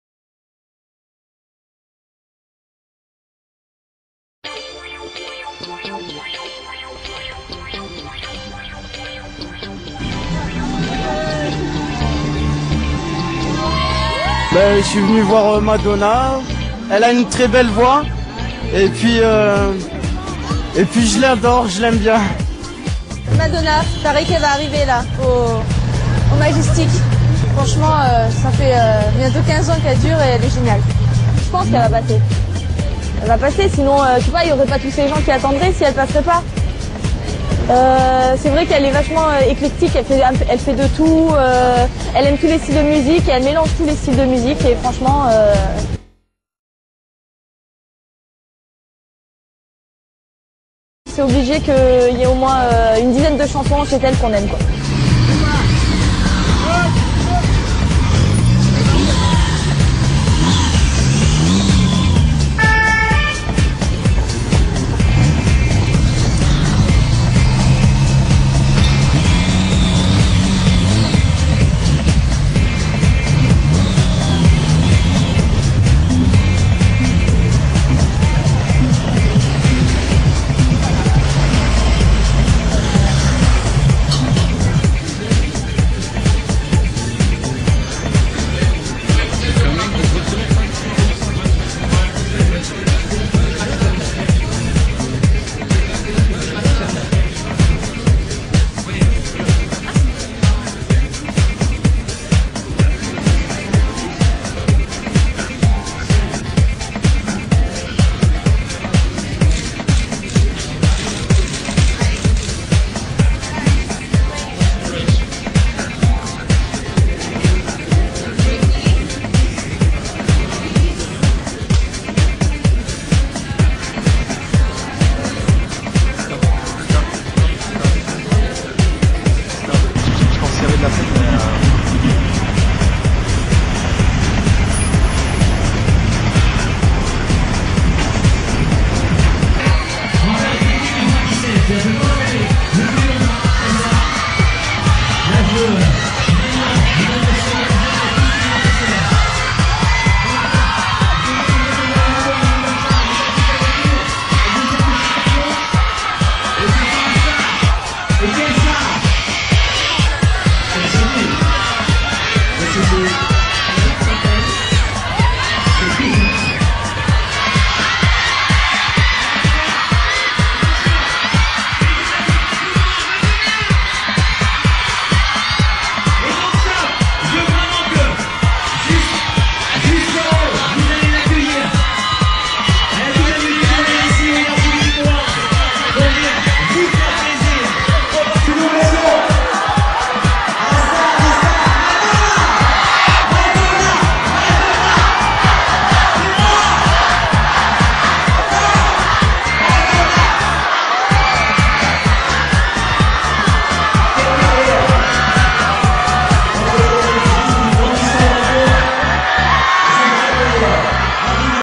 The Queen of Pop: Madonna NRJ Awards Arrival
Queen of Pop Madonna arriving at the NRJ awards 2003.